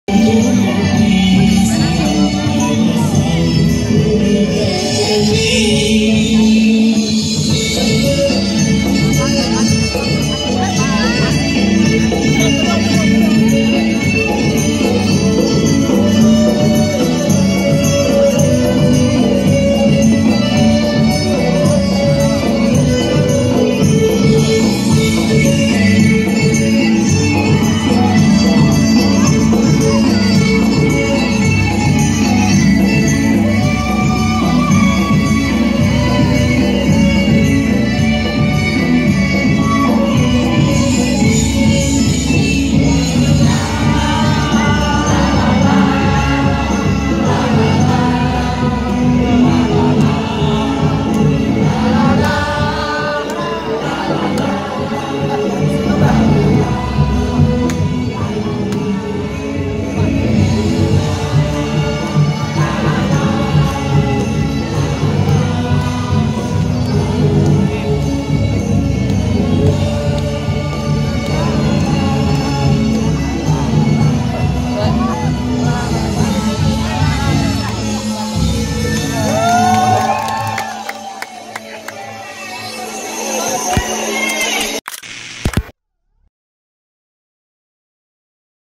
Pelajar Babel Pecahkan Rekor Dunia MURI Tari Campak Terbanyak
BANGKAPOS.COM, BANGKA - 2.000 Pelajar SMA/SMK se Provinsi Kepulauan Bangka Belitung memecahkan rekor MURI Tari Campak Terbanyak yang ditarikan oleh Pelajar dari tujuh kabupaten/kota se Babel.

Tari ini menceritakan tentang pergaulan muda-mudi di Babel, yang menggambarkan tentang kegembiraan para muda mudi di Babel. (Bangka Pos/Krisyanidayati)